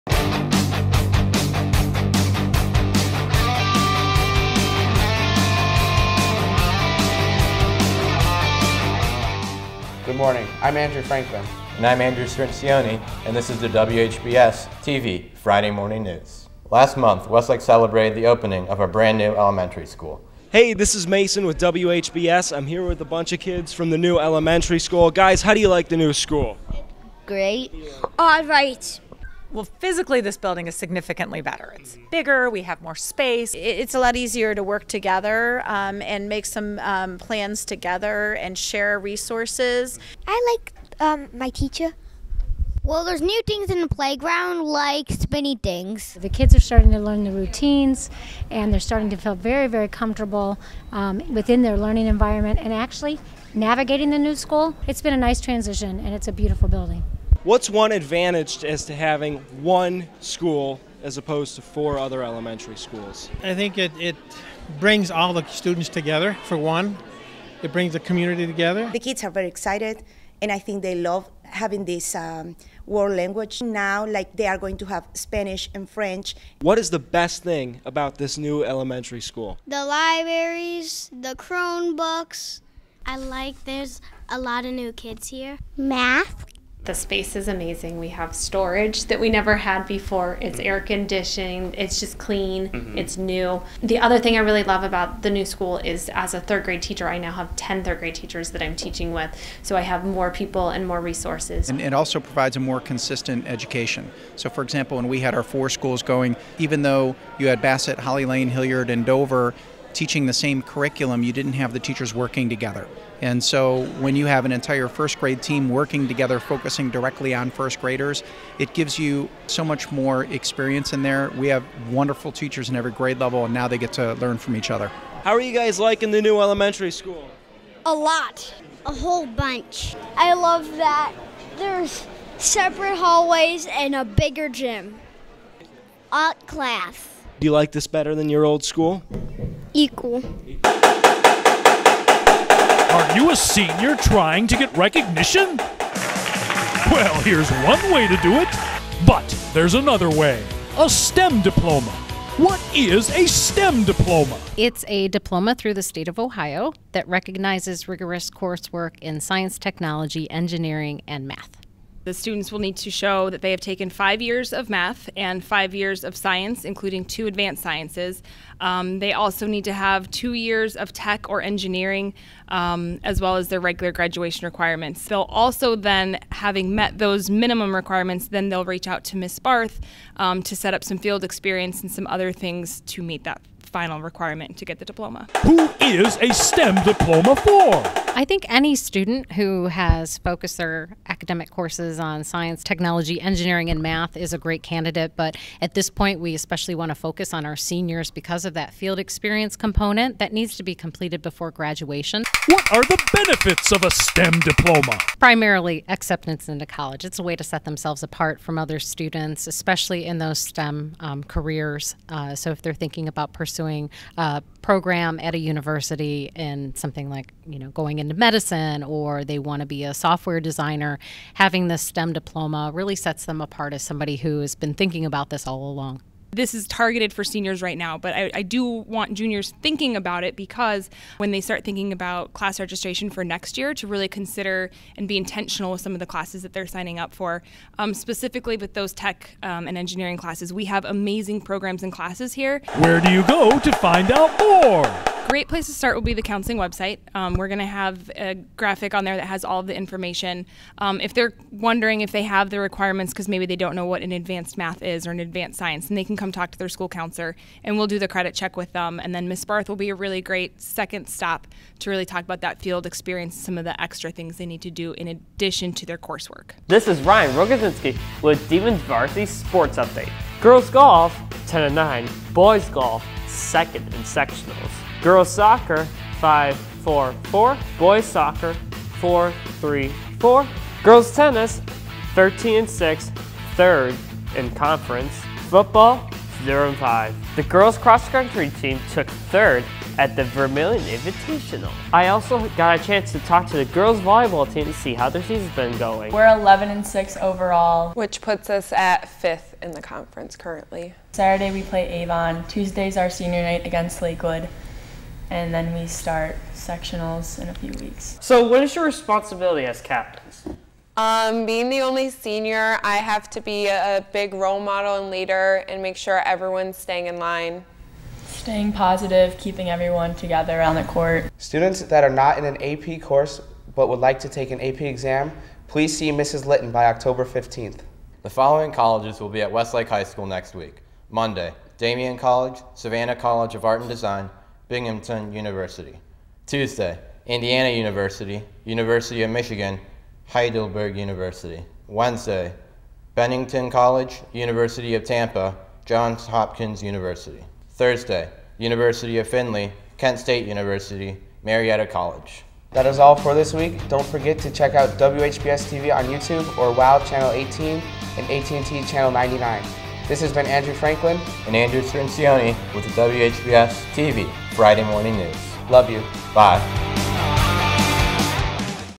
Friday Morning News 10 4 2019
This episode contains a story on the new Westlake Elementary School, a discussion on the STEM Diploma, and an interview with the Volleyball Team captains.